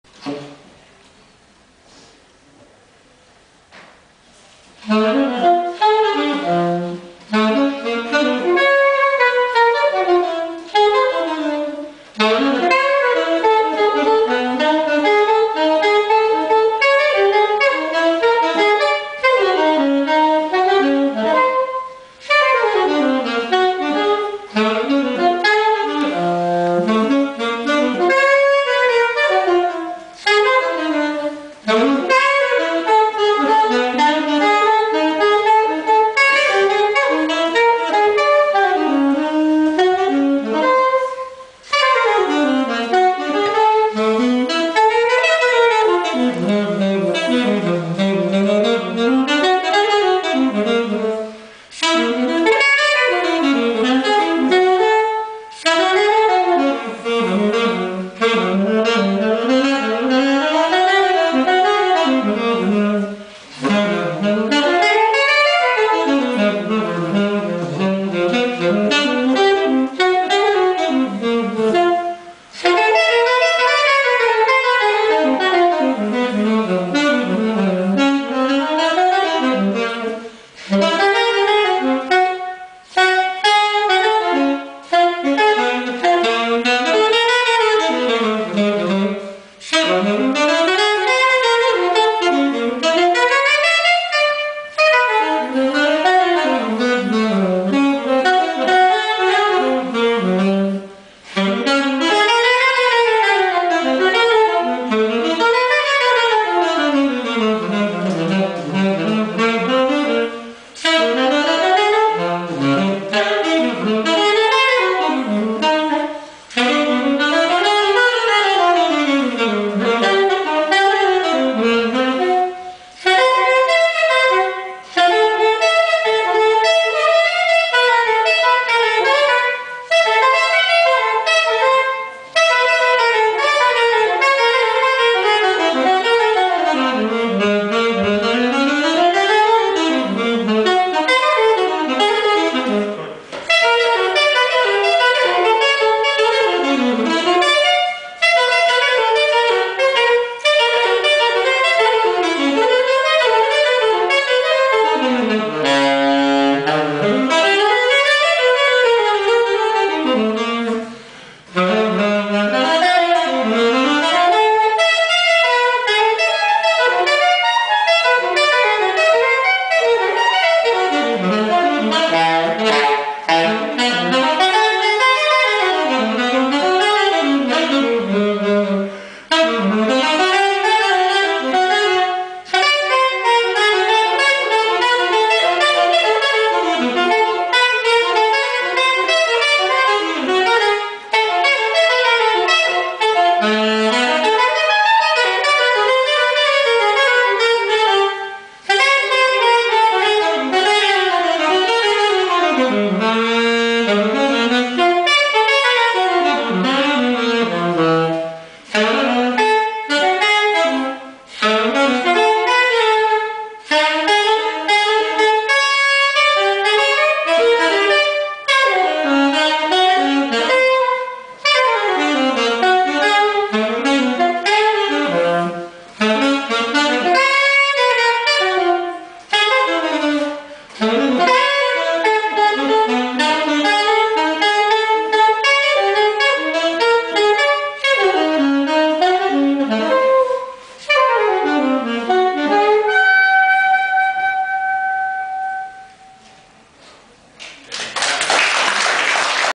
Frank Gratkowski plays Albert Mangelsdorff
FG LIVE @ The White House 03/15/2009...playing a tune from the late great Albert Mangelsdorff. check out for info about upcoming concerts, etc.